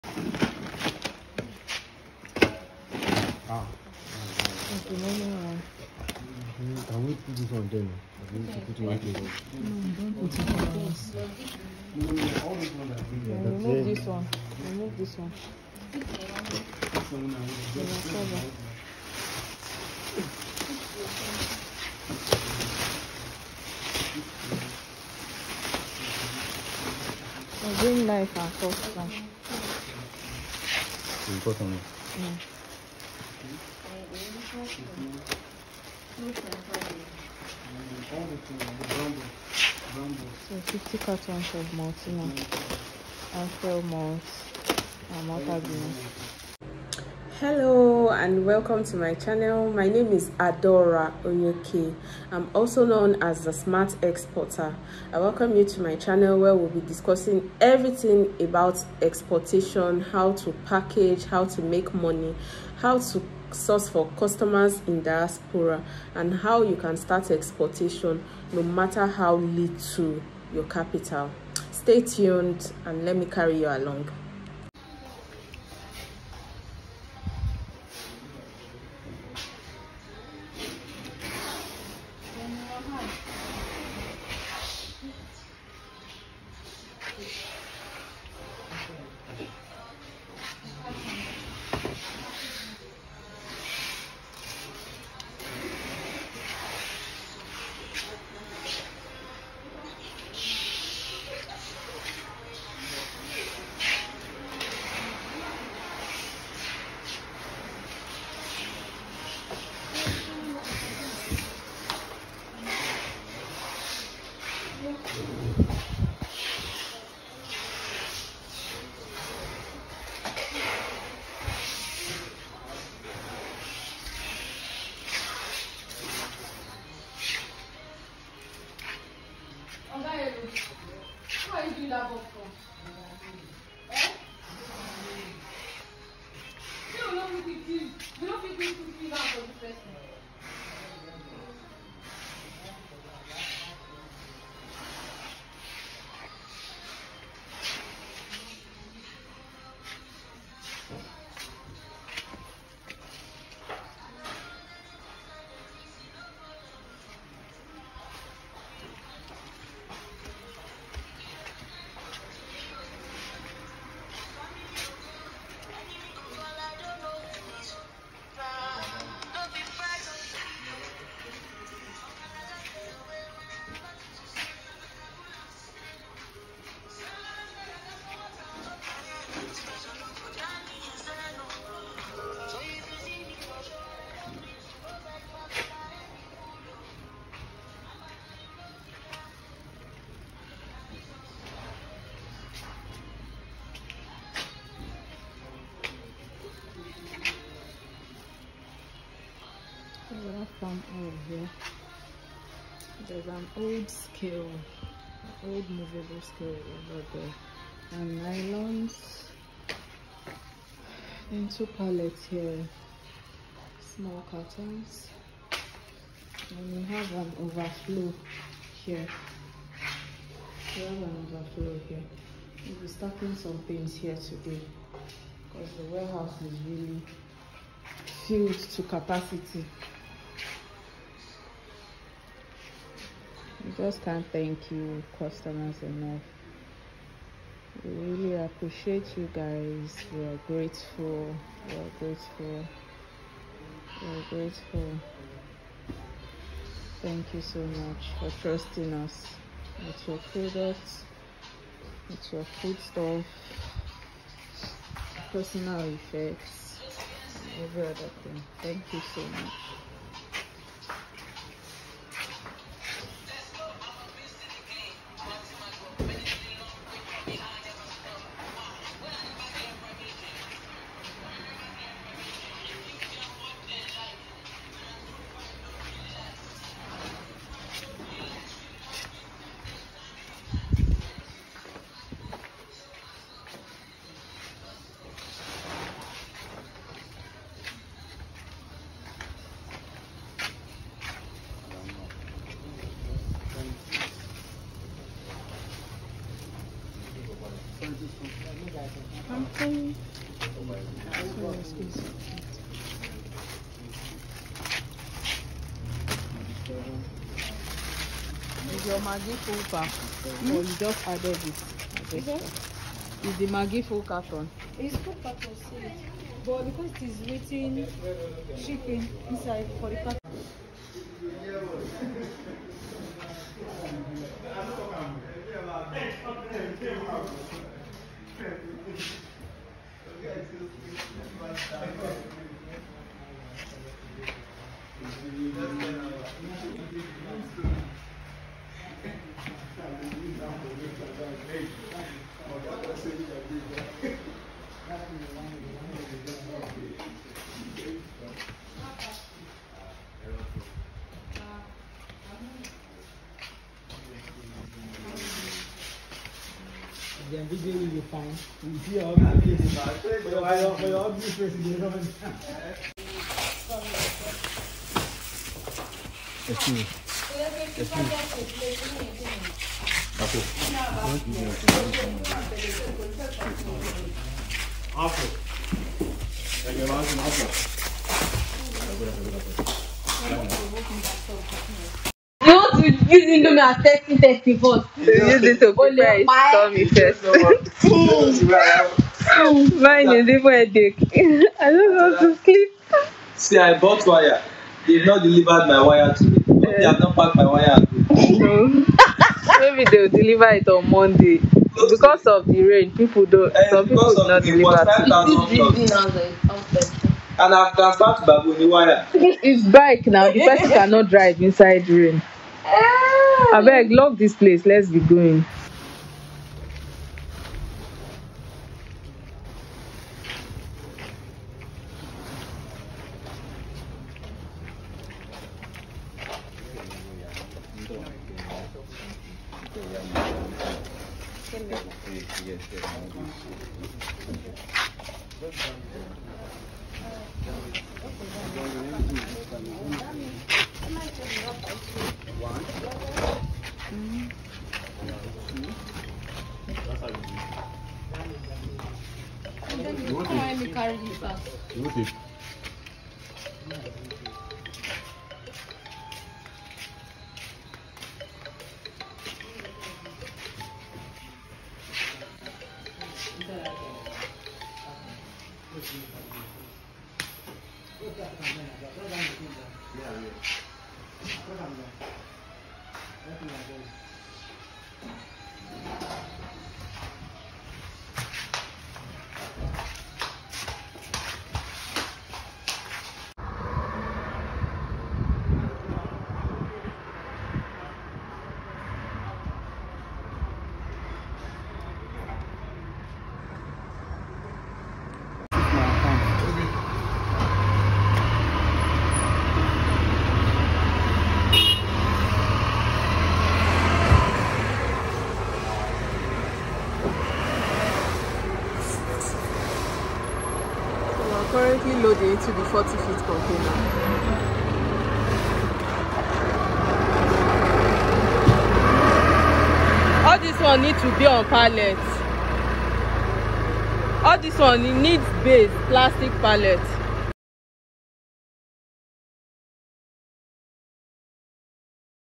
CARGO COLLECTION FOR GROUP SEA CARGO// HOW GROUPAGE WORKS.
PACKAGING GOODS FOR EXPORTS VIA SEA CARGO \\ DO NOT BE INTIMIDATED BY THE LARGE Quantity YOU See in the video . A lot of the items came in as small packages, 50kg, 100kg from mini exporters. Our job at the warehouse is to take inventory, package the goods in the best way and pool them together to meet the volume required for a full container.
If you have more questions , kindly drop them in the comment section I’ll attend to all your questions.